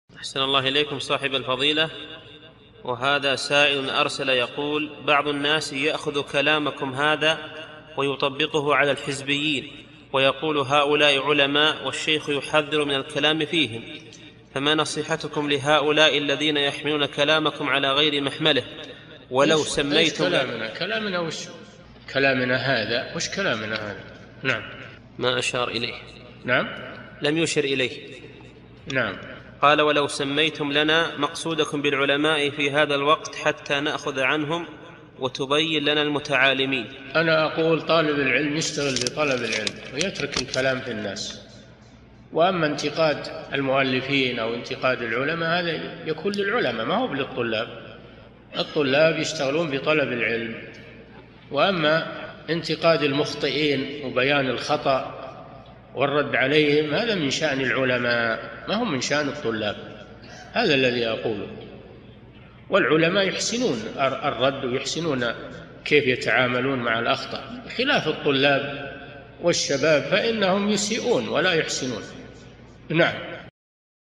[17 -954] البعض يأخذ كلامكم هذا ويطبقه على الحزبيين ويقول هؤلاء علماء والشيخ يحذر من الكلام فيهم..
-[17 /954] السؤال: بعض الناس يأخذ كلامكم هذا ويُطبقه على الحزبيين ويقول: هؤلاء علماء والشيخ يُحذر من الكلام فيهم، فما نصيحتكم لهؤلاء الذين يحملون كلامكم على غير محملِهِ، قال: ولو سميتم لنا مقصودكم بالعلماء في هذا الوَقْت حتى نأخذ عنهم، وتُبين لنا المتعالمين؟
الجواب: أنأ أقول: طالب العلم يشتغل بطلب العلم ويترك الكلام في الناس، وأما انتقاد المؤلفين، أو انتقاد العلماء، هذا يكون للعلماء ما هو للطلاب، الطلاب يشتغلون بطلب العلم، وأما انتقاد المخطئين وبيان الخطأ والرَّد عليهم، هذا من شأن العلماء ما هو من شأن الطلاب، هذا الذي أقولُهُ، والعلماء يُحسنون الرد ويُحسنون كيف يتعاملون مع الأخطاء، بخلاف الطلاب والشباب فإنهم يُسيئون ولا يُحسنون.
للاشتراك في قناة الشيخ صالح الفوزان
فتاى شرح منظومة الآداب ( 945 ) مقطع

للاستفسار والإقتراحات والدعم
رئيس الفريق
لمشروع كبار العلماء